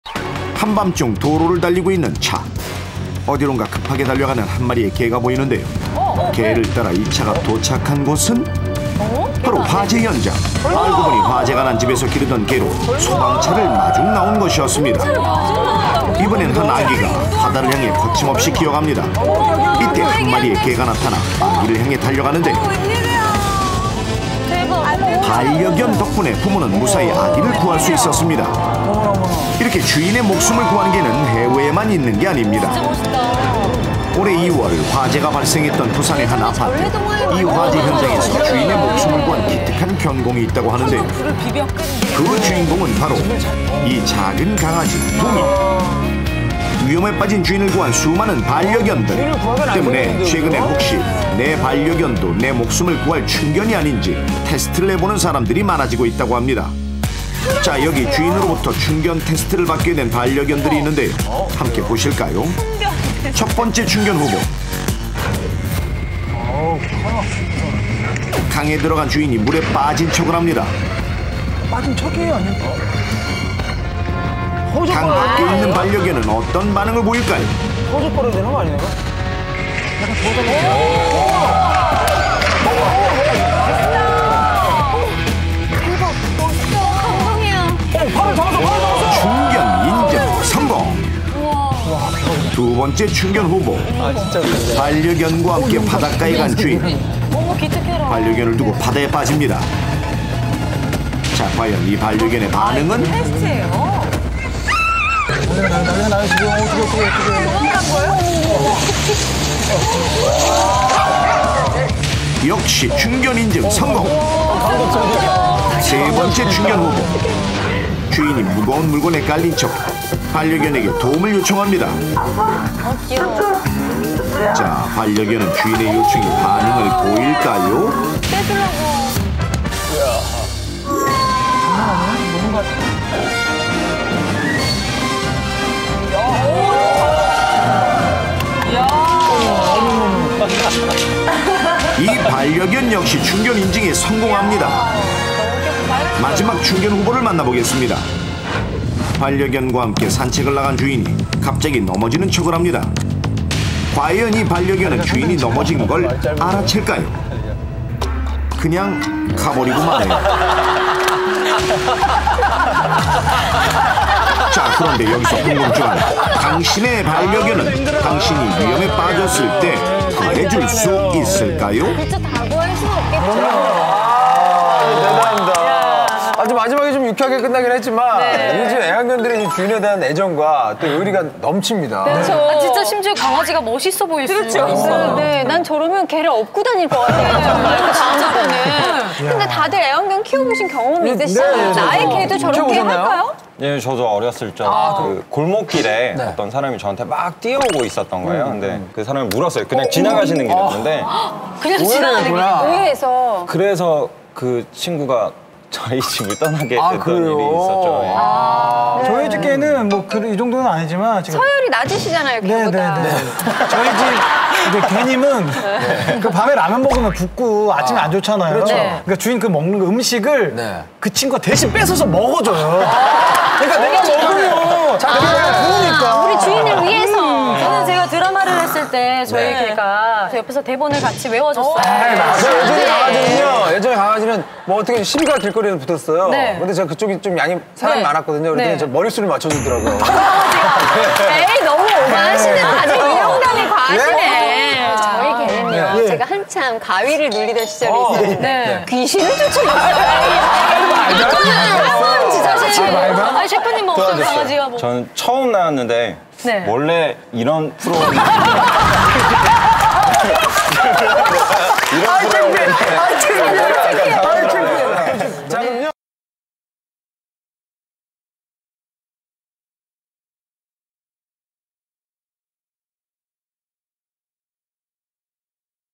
[감동주의] 주인이 물에 빠졌을 때 충견의 반응 Dog's reaction when the owner falls into the water
주인을 구하기 위한 반려견들의 행동들은 너무나 감동적입니다!

The dogs' actions to save their owners are very touching.